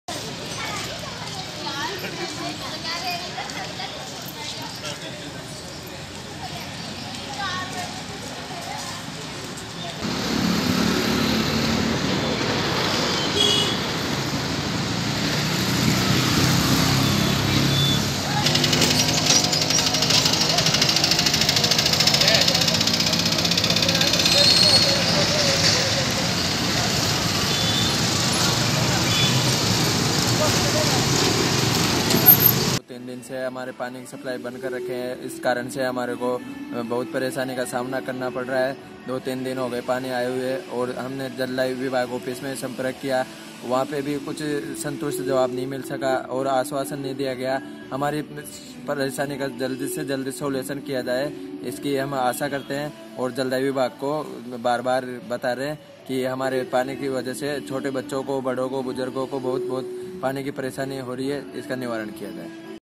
जल संकट से जूझ रही गुलाबी नगरी, नहीं मिल रहा लोगों को पानी
जयपुर.बीसलपुर बांध में कम पानी और पंप हाउस की सफाई के चलते गुलाबी नगरी के बाशिंदे पानी के संकट से जूझ रहे हैं.दो दिन से उन्हें पानी नसीब नहीं हो पा रहा है.शुक्रवार को बाला वाला और शनिवार को रामनिवास बाग पंपहाउस के ट्रांसफार्मर में तकनीकी खराबी के चलते गुलाबी नगरी के लोगों को पानी नहीं मिला.इसके कारण उन्हें काफी परेशानी का सामना करना पड़ा.